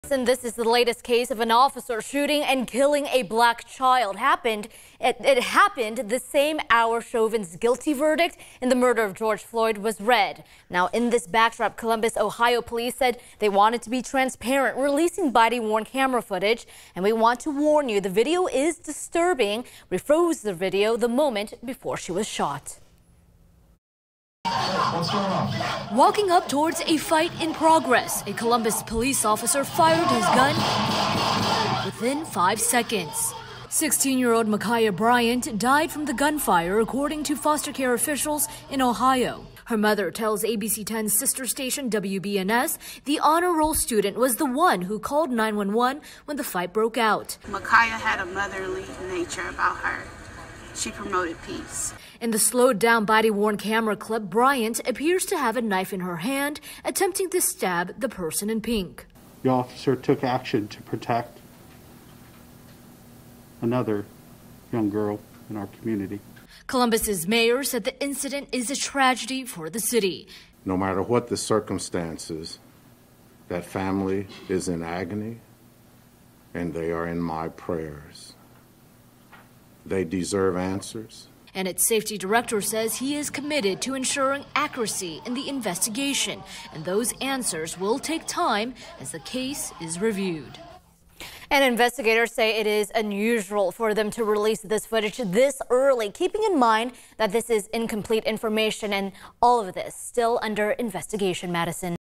Ohio police shoot 16-year-old girl on same day as Derek Chauvin verdict
According to the 16-year-old's mom, she was the one who called the police when a fight broke out in her neighborhood.